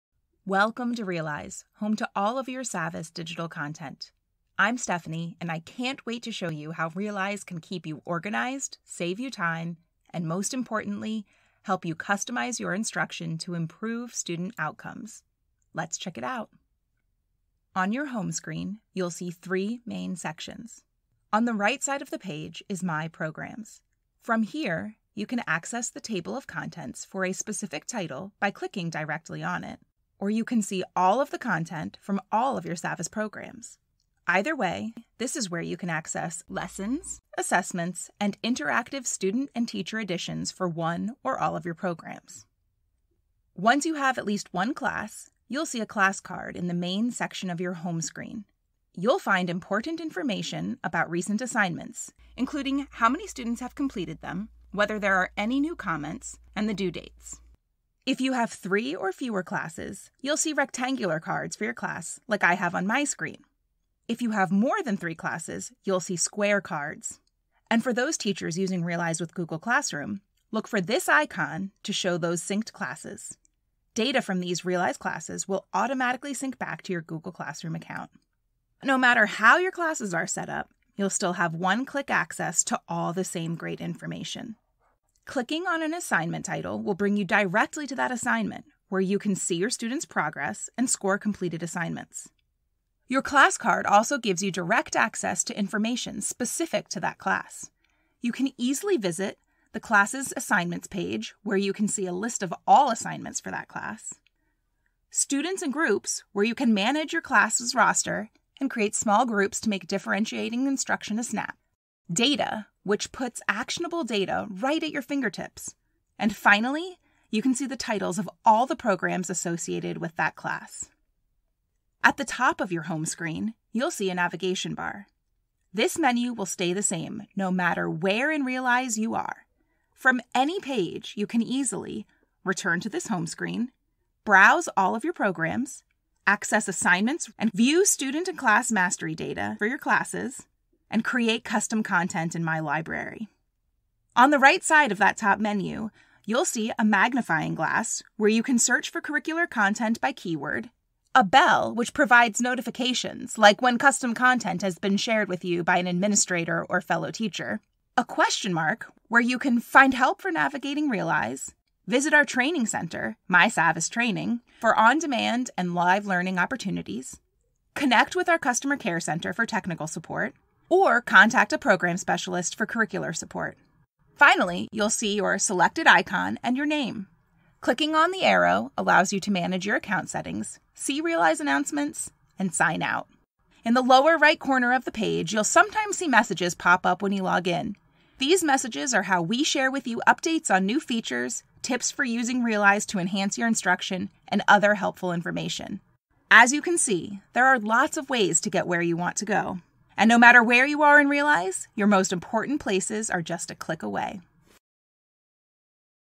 Getting Started with Savvas Realize
See how our award-winning platform, Savvas Realize, helps to keep you organized, save you time, and most importantly, customize your instruction to improve student outcomes! New to Realize? Request a demo today